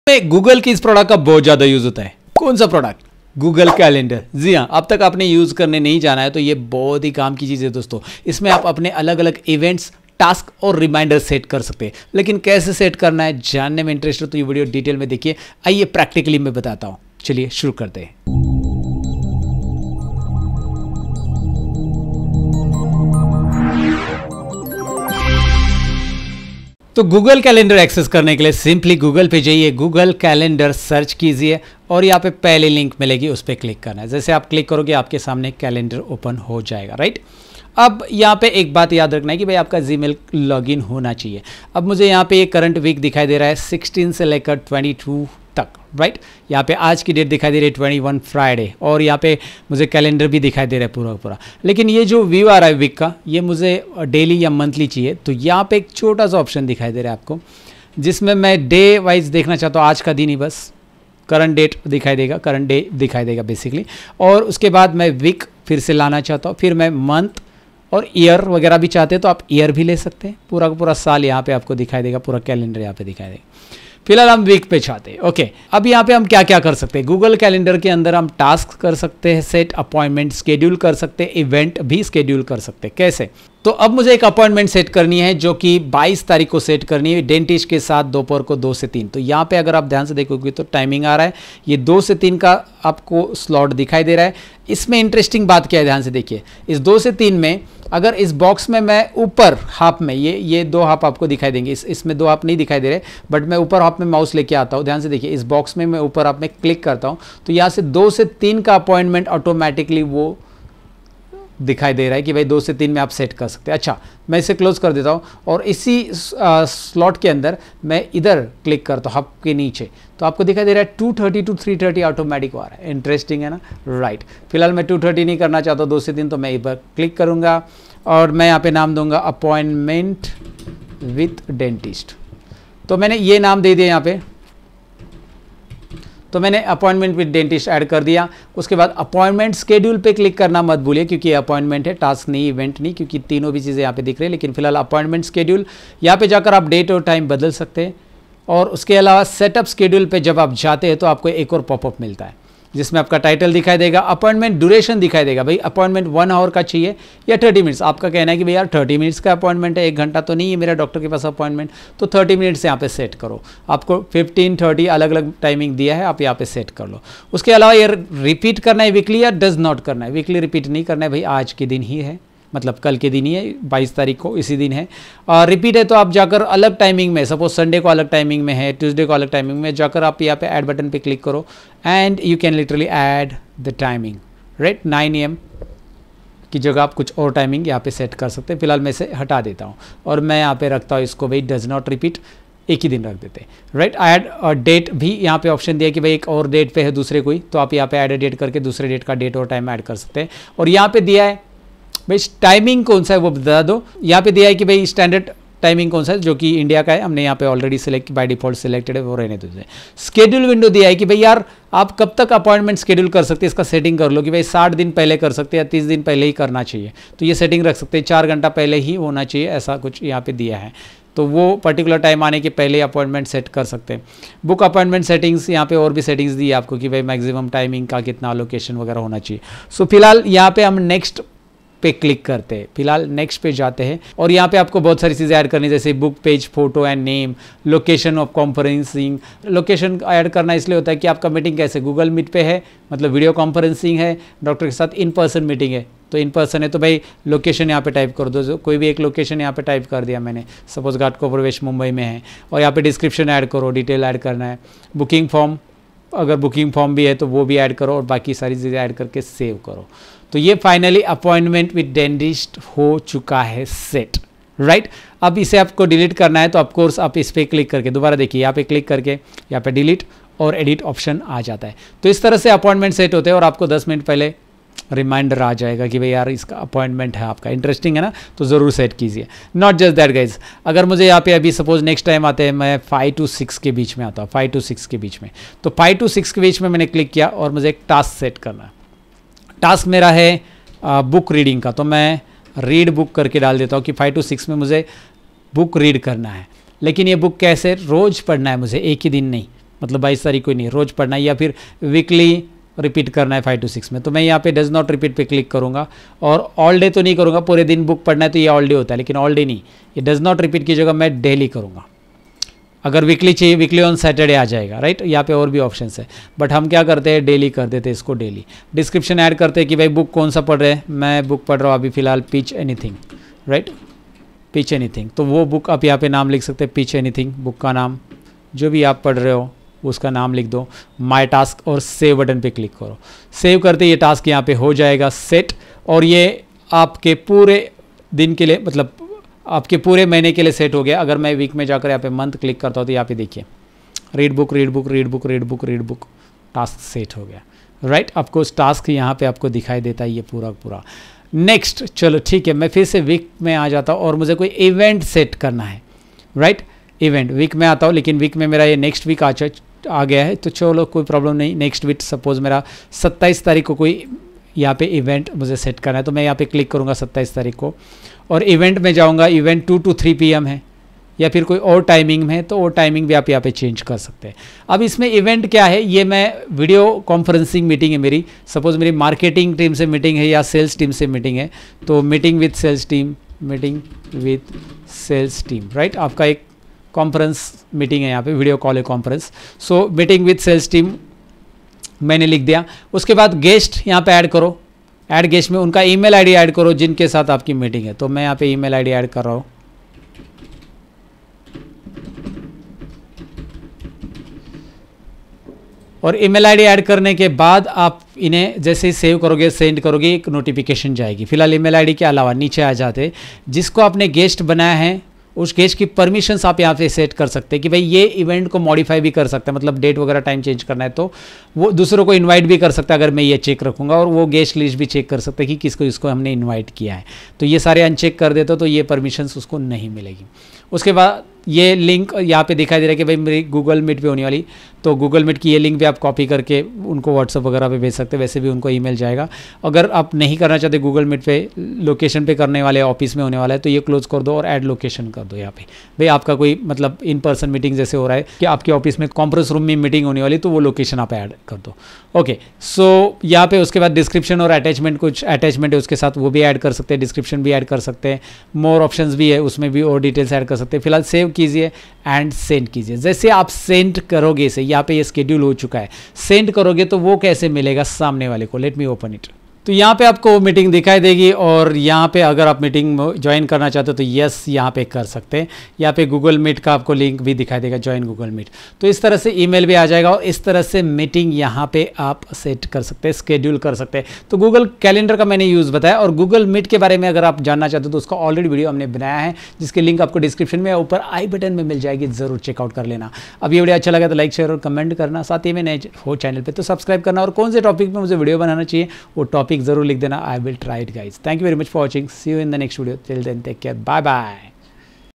How to Setup Meeting, Event, Task in Google Calendar. Learn Google Calendar in 8 Minutes.
Course Features :
✅ PDF Shortcuts
✅ Course Completion Certificate
✅ Instant Access